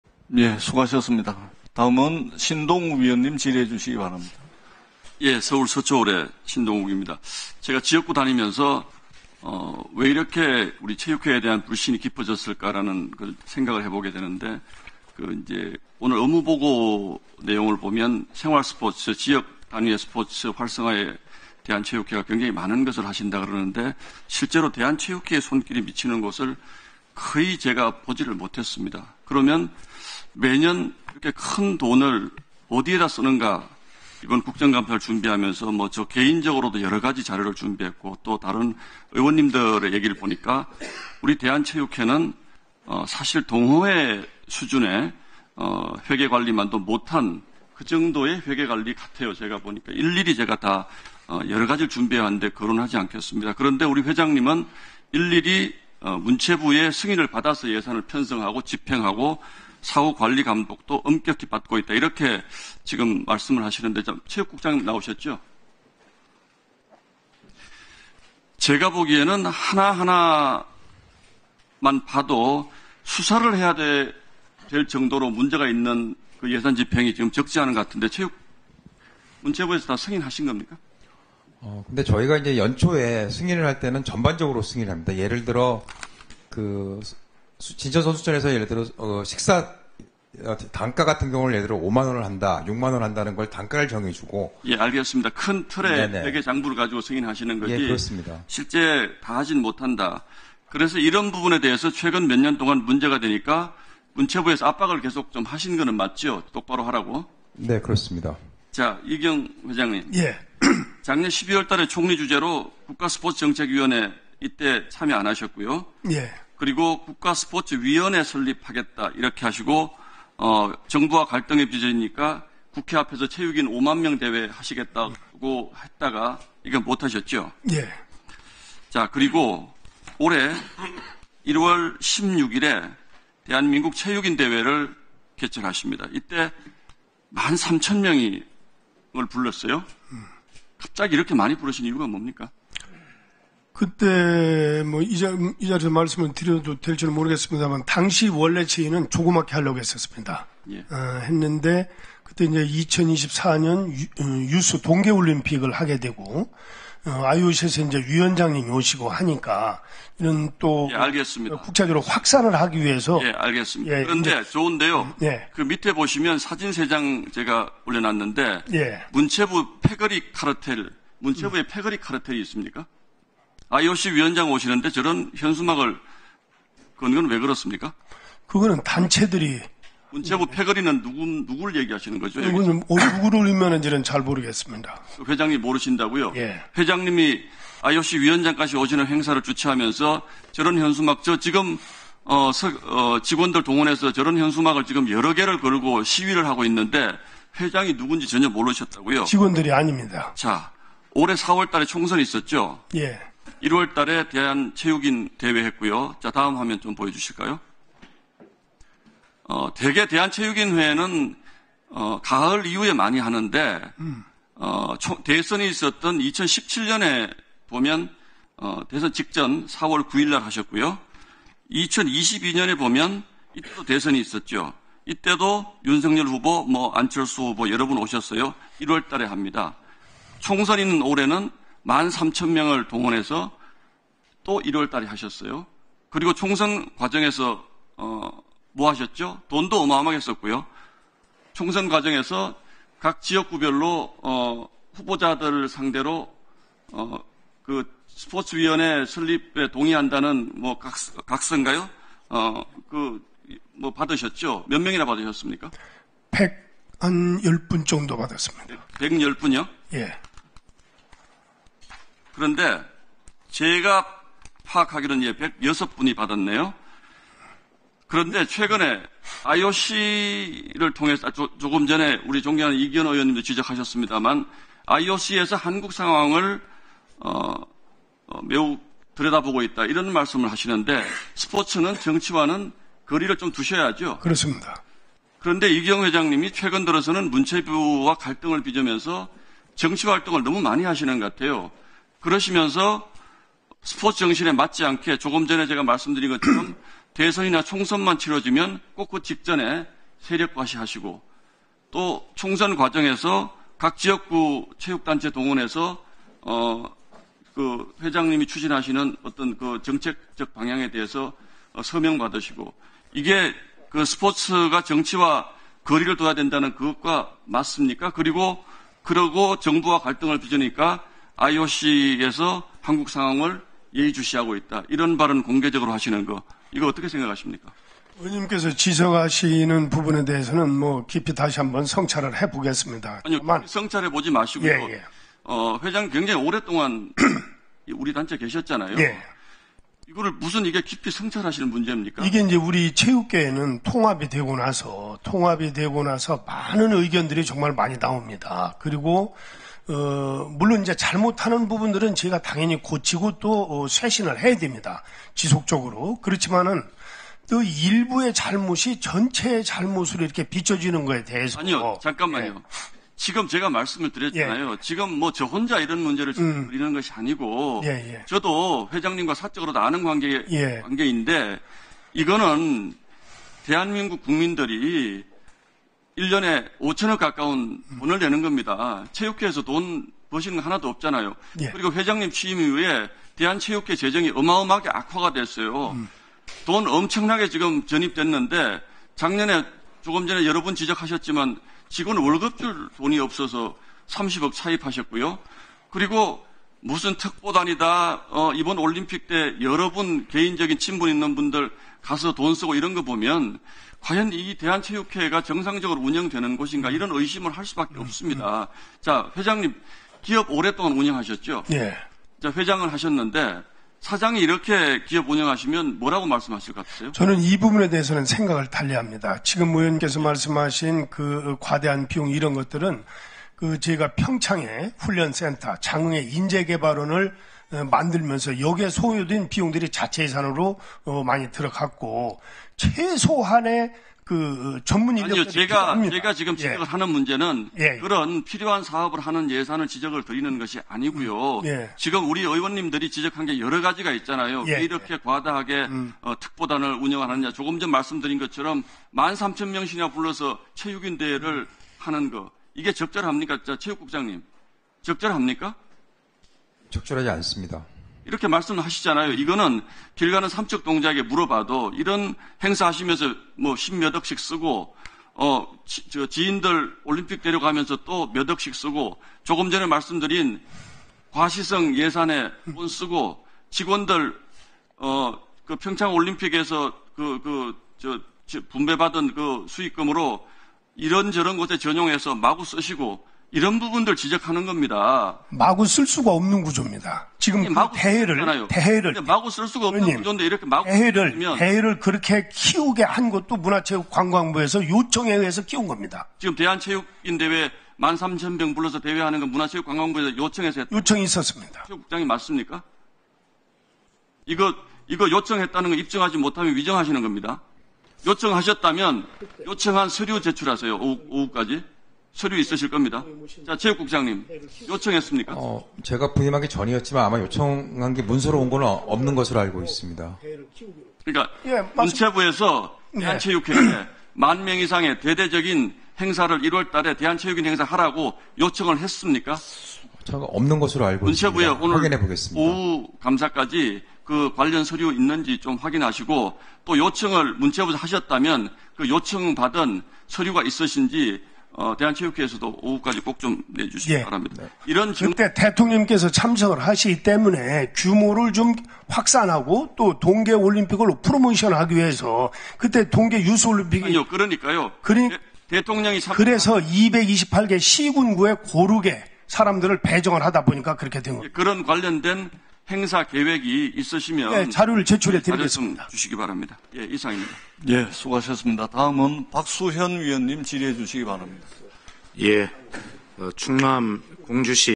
"체육회 회계는 동호회 수준"... 마구잡이 예산 낭비 뼈 때린 신동욱
국회 문체위-대한체육회 등 국정감사 (24.10.22)